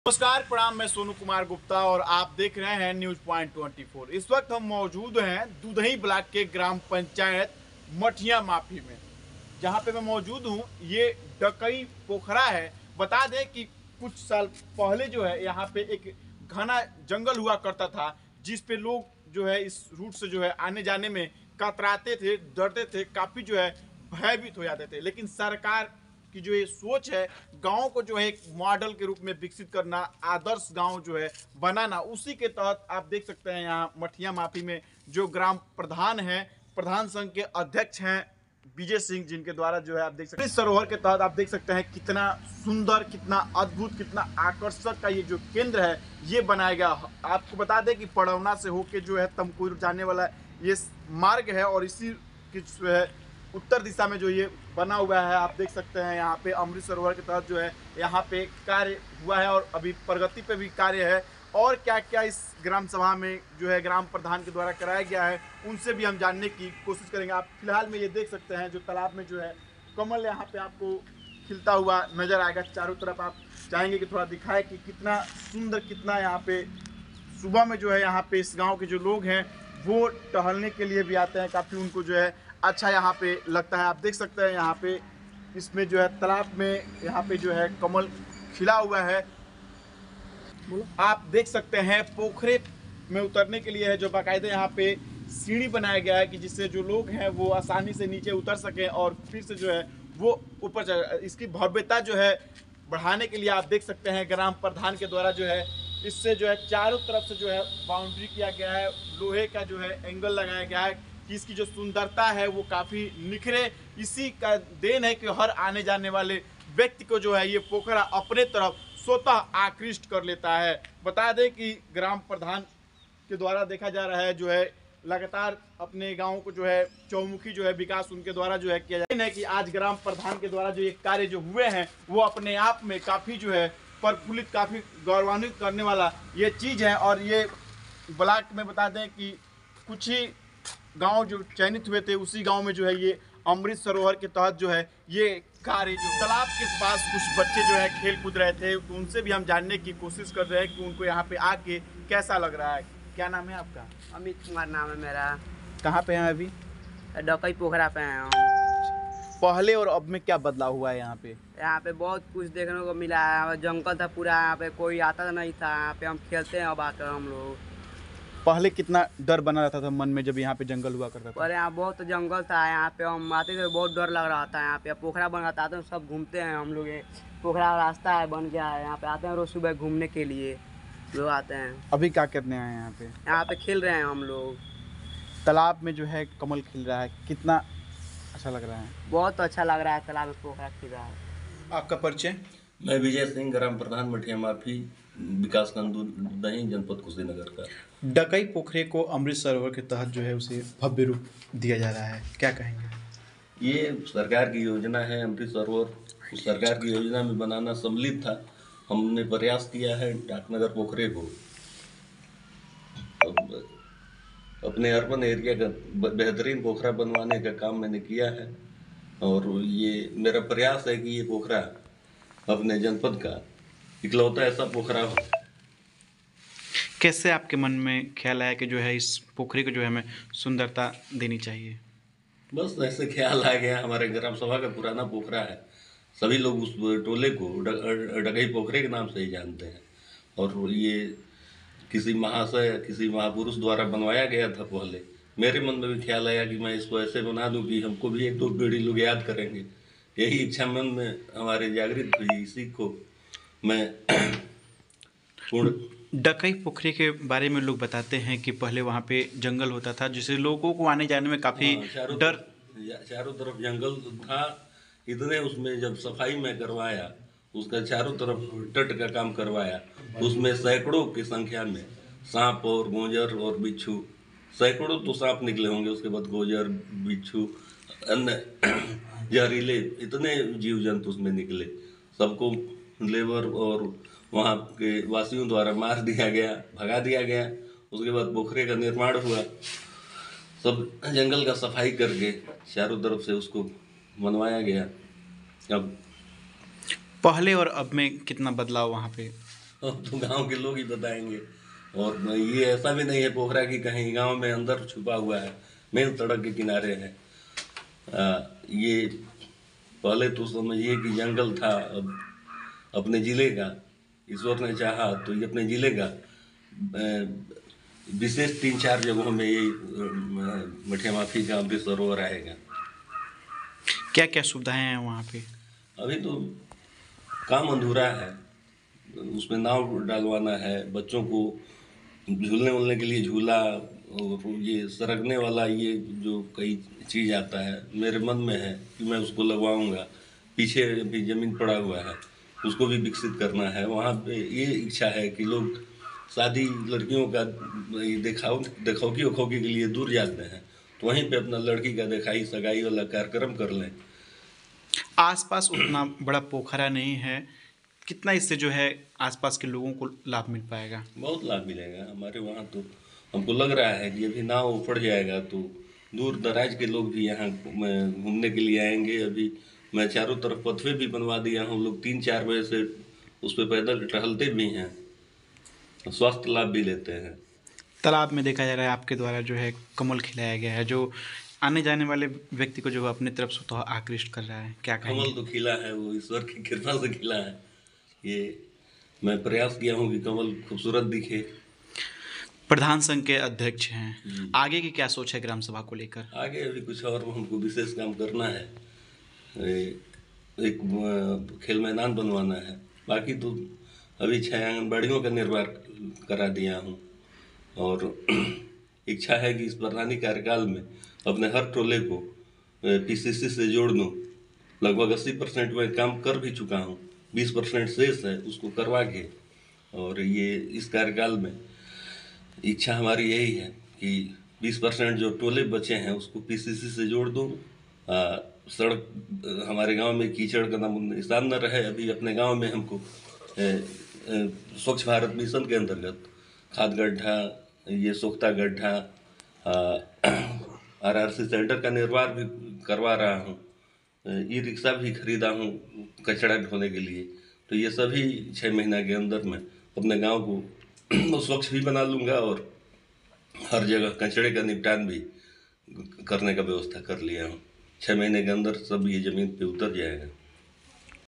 kushinagar कभी डकही पोखरे का नाम सुनते ही सहम जाते थे राहगीर,आज पोखरे....
कभी डकही पोखरे का नाम सुनते ही सहम जाते थे राहगीर,आज पोखरे पर खिला कमल

कुशीनगर जनपद के दुदही विकासखंड के ग्राम पंचायत मठिया माफी में अमृत सरोवर परियोजना के तहत पोखरे को ग्राम प्रधान विजय सिंह के द्वारा कराए जा रहा है भव्य सुंदरीकरण।